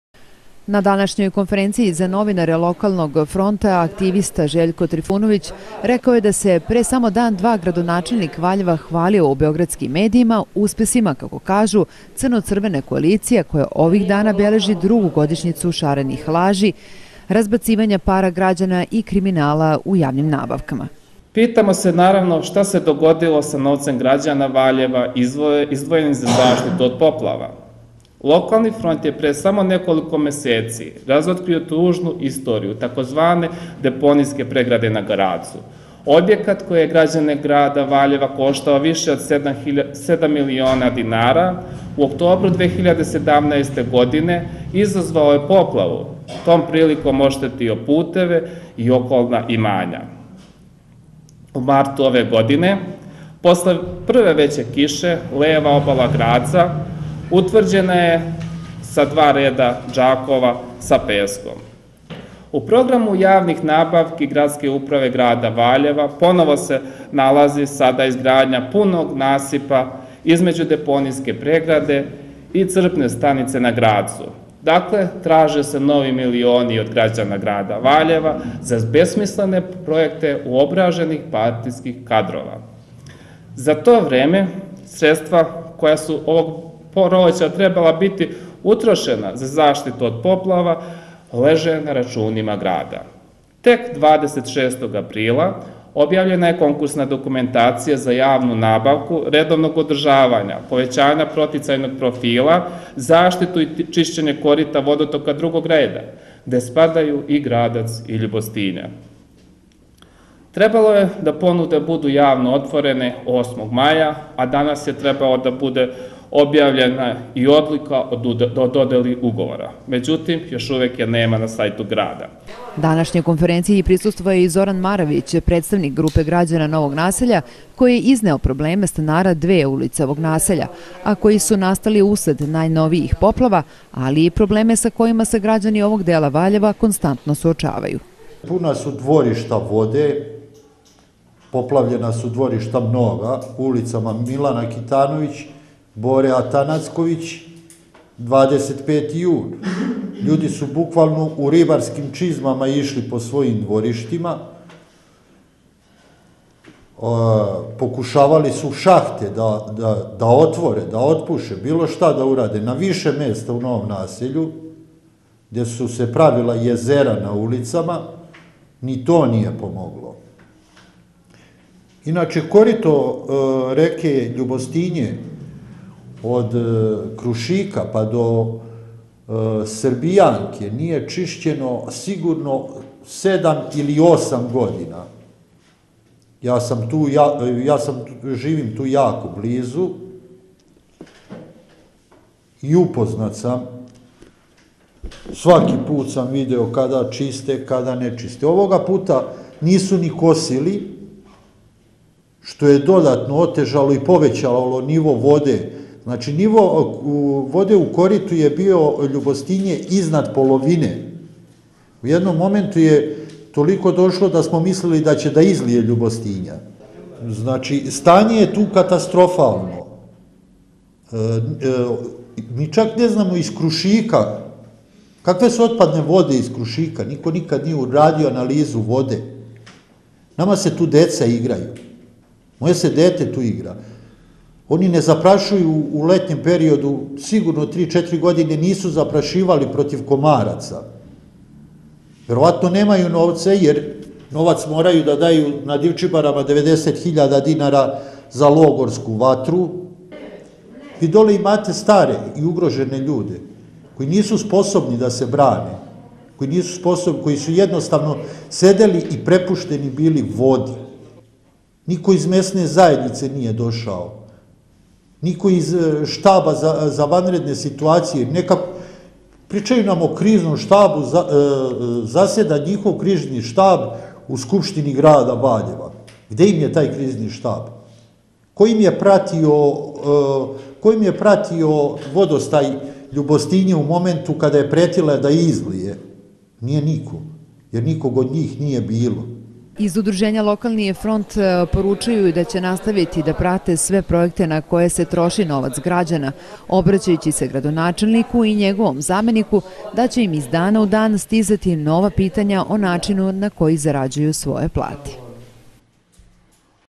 Valjevo na vodi
Konferencija za medije LF VA o stanju u kojem grad Valjevo dočekuje visoke padavine, nakon bolnih iskustava u nedalekoj prošlosti.